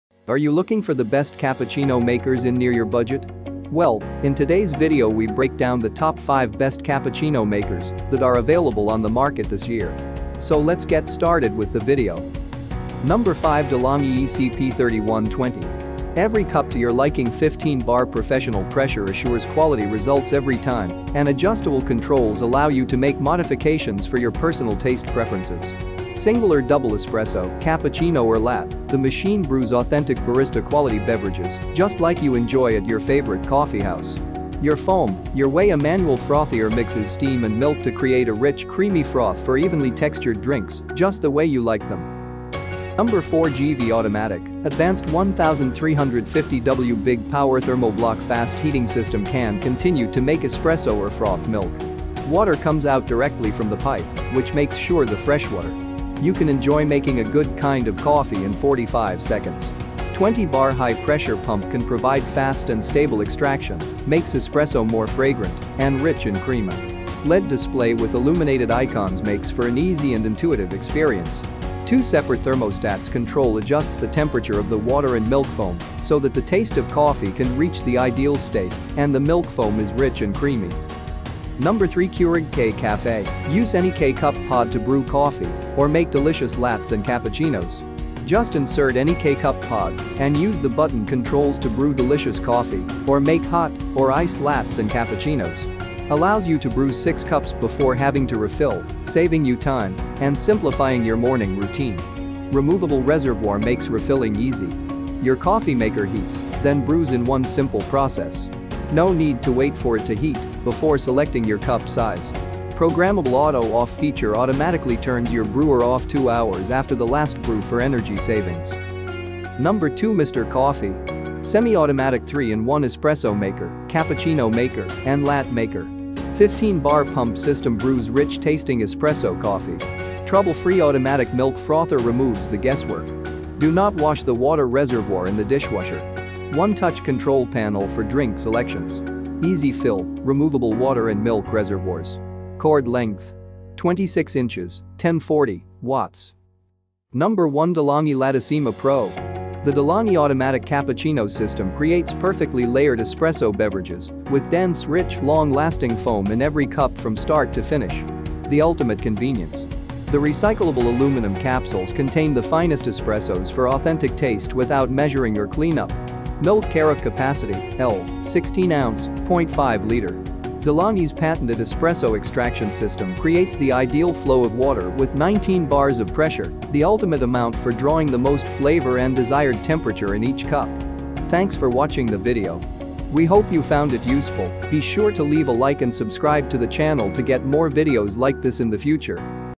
Cappuccino Makers : Top 5 Best Cappuccino Makers 2021
Are you looking for the best cappuccino makers? You can watch the top 5 best cappuccino makers reviews. Follow below for more details.

1. DeLonghi Lattissima Pro EN750MB

2. Mr. Coffee Espresso and Cappuccino Maker

3. Keurig K-Cafe Cappuccino Maker

4. Gevi Automatic Cappuccino Coffee Maker

5. De'Longhi ECP3120 Cappuccino System